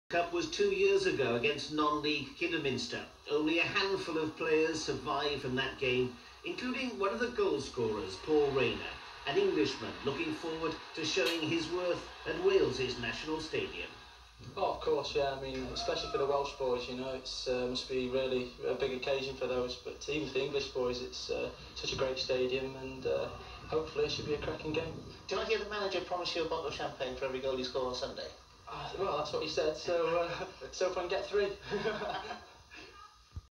Paul Raynor interview before 1991 welsh cup final. Swansea City.
Paul Raynor being interviewed at the vetch before the 1991 welsh cup final v Wrexham at Cardiff Arms Park. features Paul's headed goal v Kidderminster in the final 2 years before. And a great admiral kit 👌